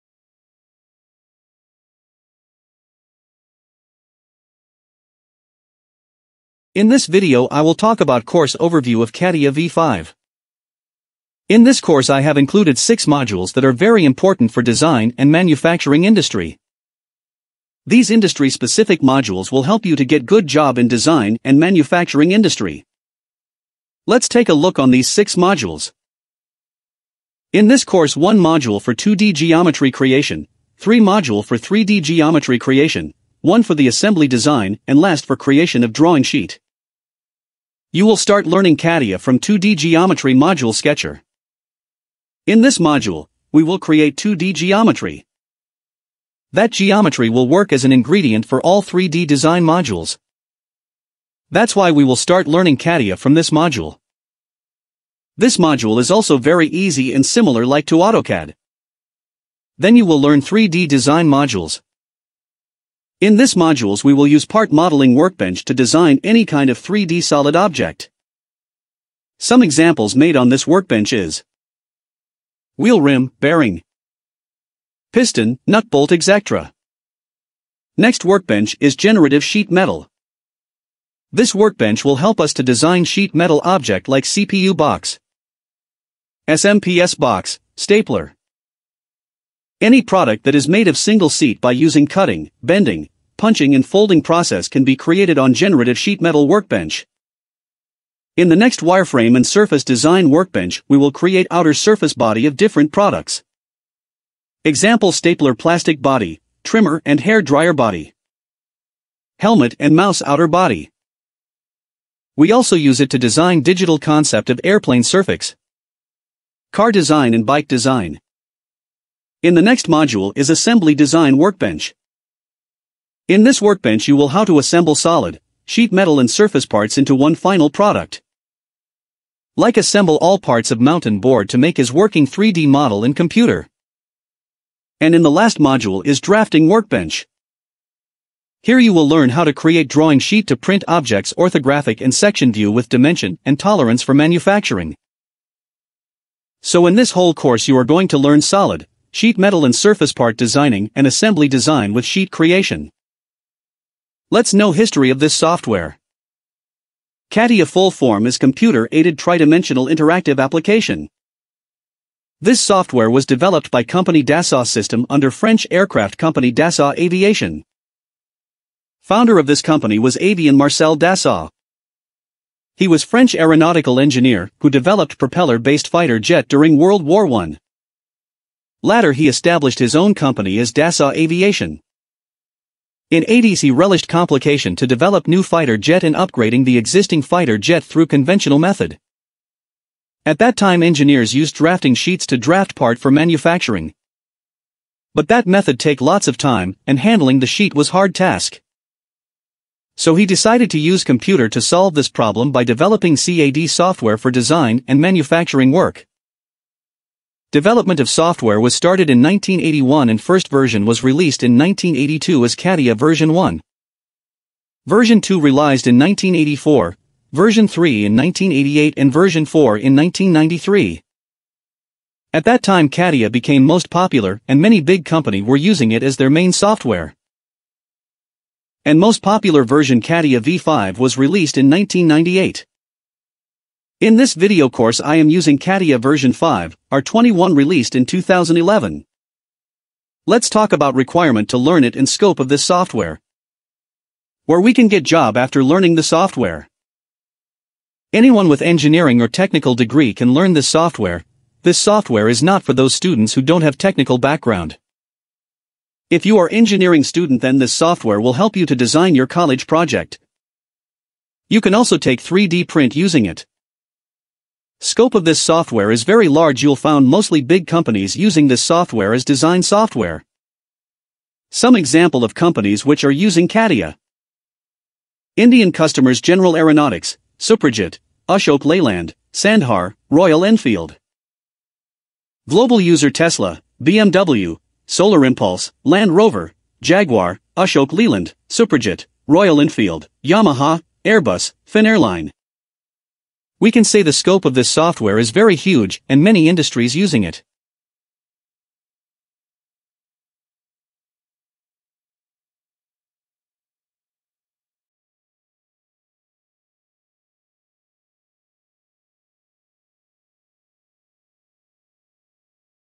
catia v5 online course introduction learn 6 modules free.
catia v5 online free course introduction.
content of this video
introduction about catia v5 course module
history of catia
founder of catia
top companies using this software.